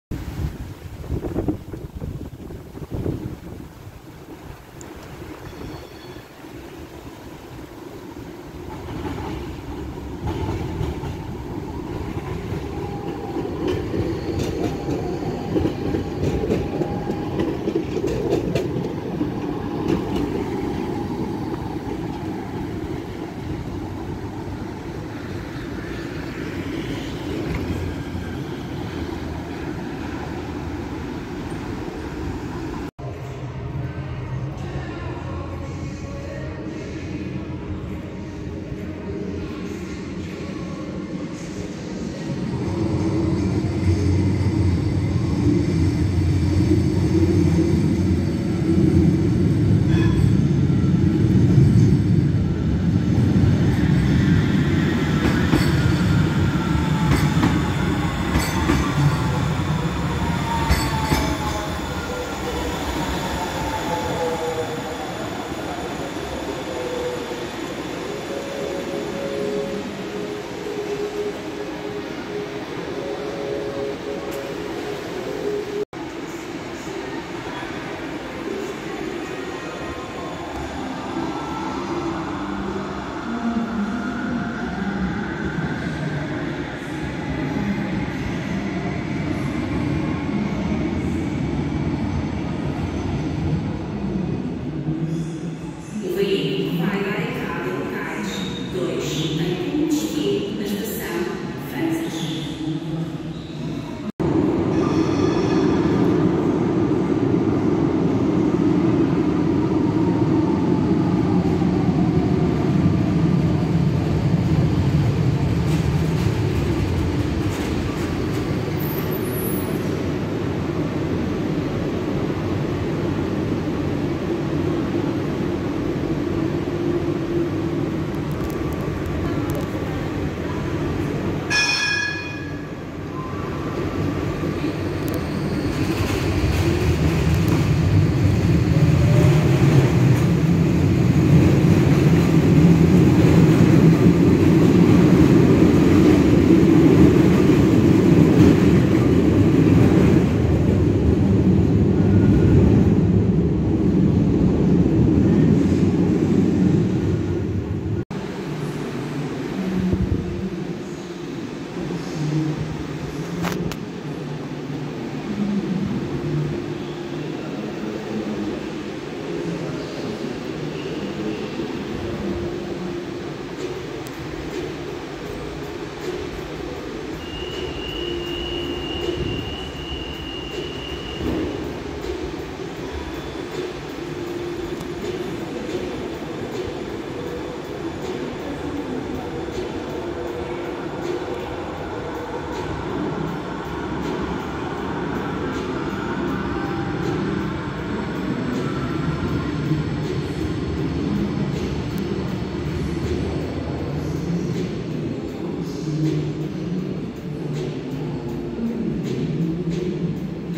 Metro do Porto em Matosinhos e Campanhã
Metro do Porto em Câmara de Matosinhos e estação de Campanhã

linhas A🔵 B🔴 C 🟢

1/10/2022